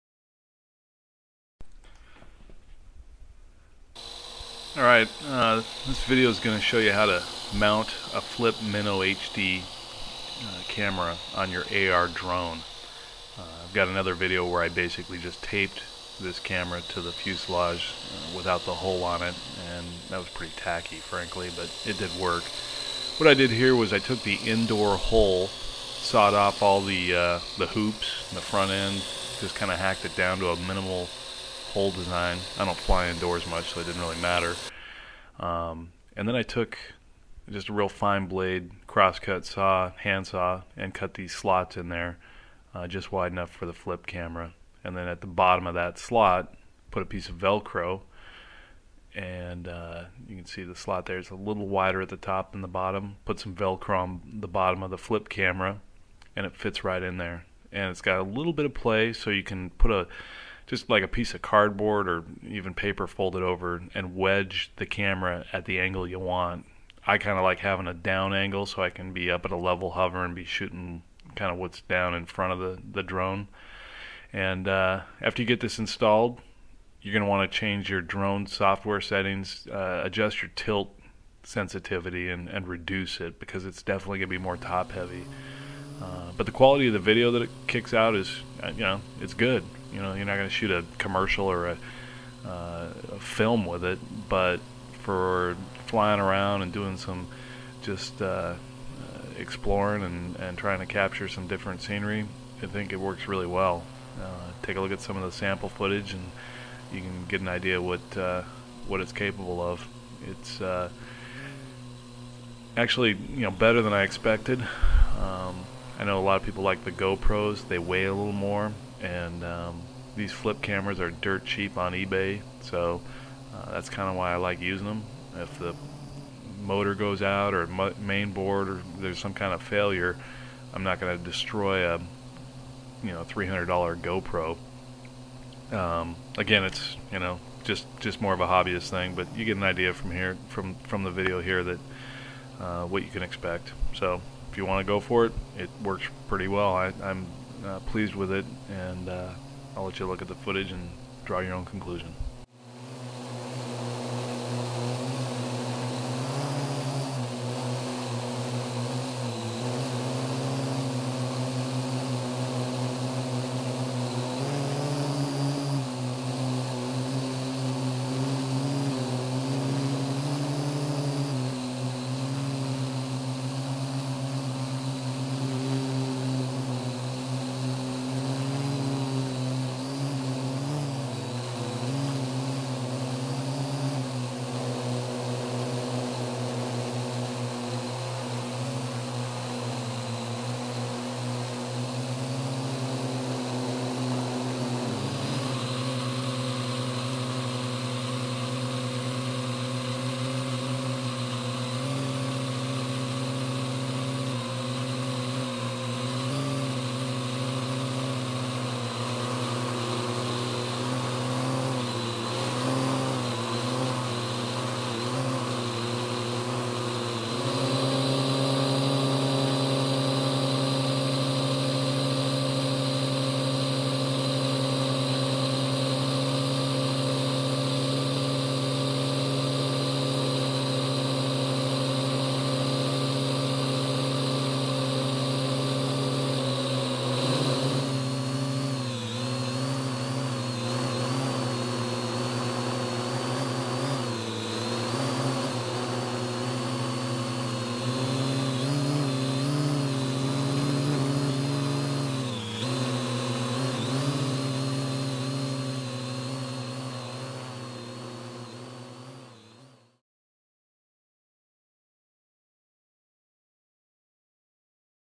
How To Mount an HD Camera on the AR Drone
This video shows how I modified the outdoor hull of the AR Drone to mount the Flip MIno HD Camcorder to it. Also has a few minutes of footage shot with this setup.